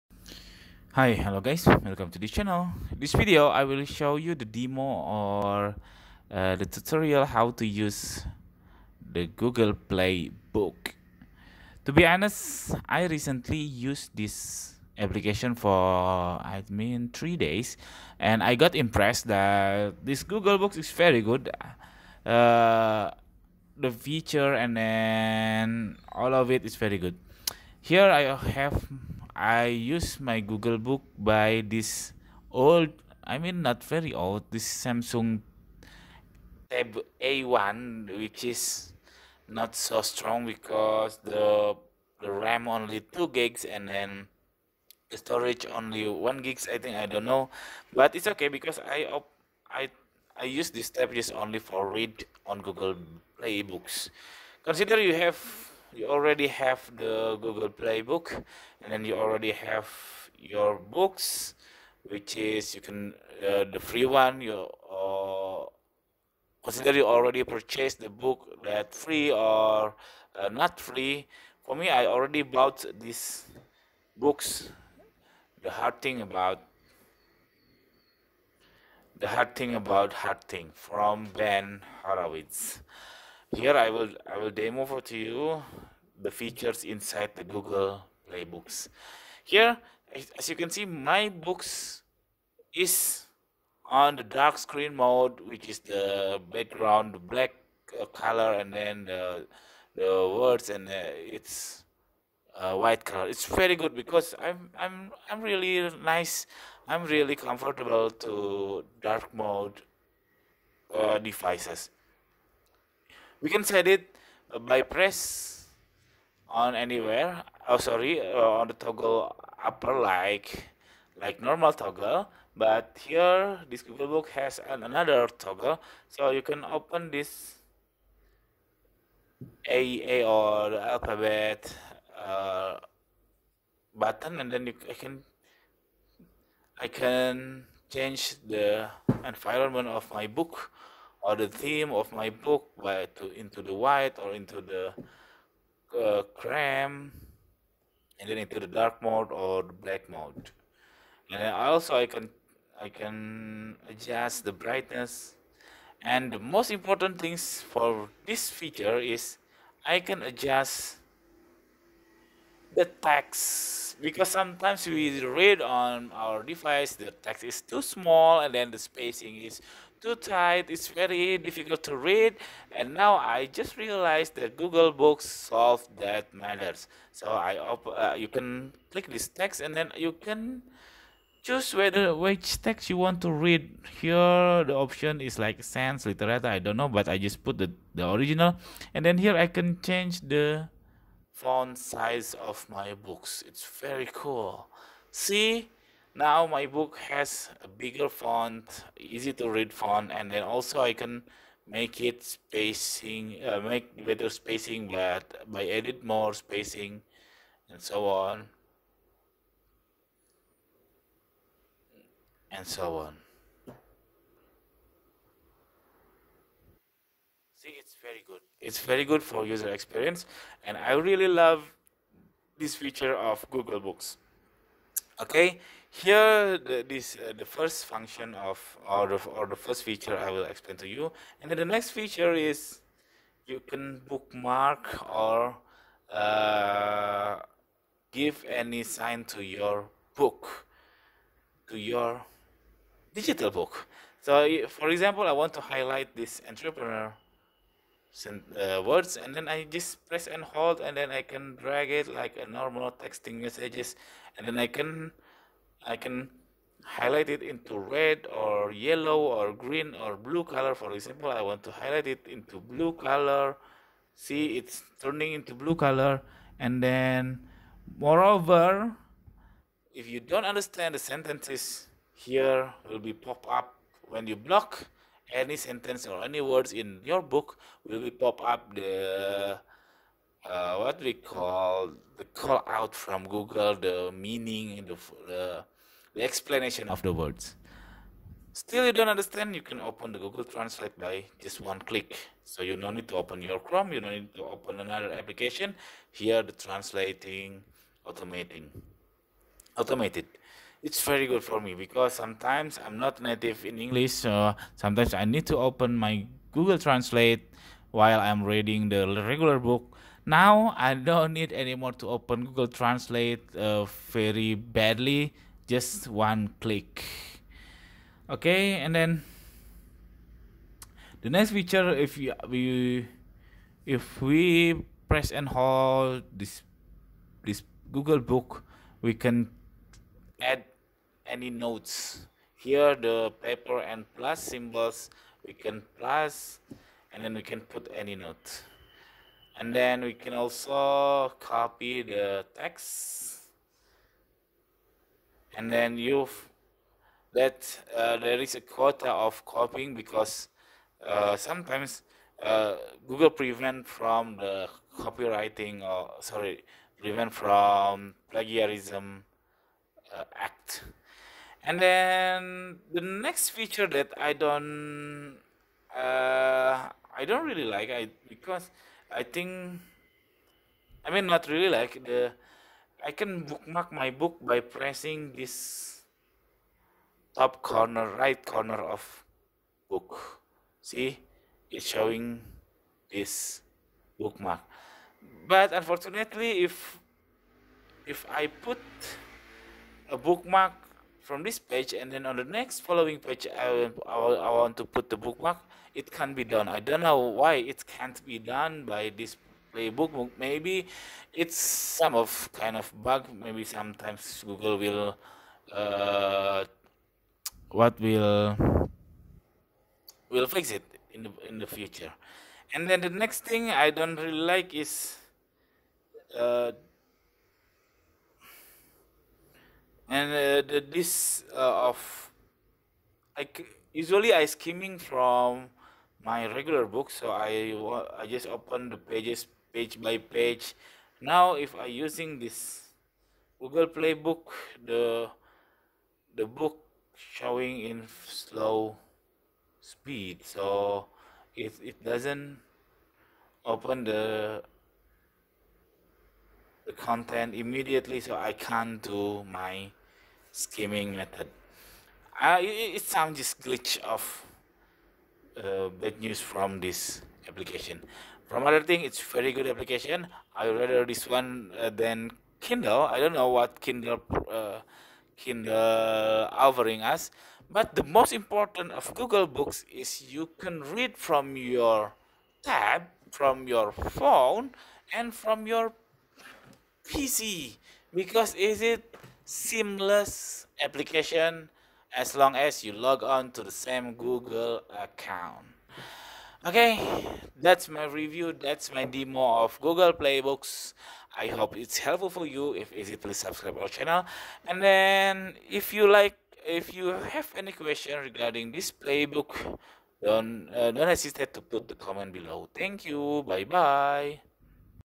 How to Use Google Play Books
This how to reading an e-book using Google Play Books apps, and my low-spec tablet Samsung A1.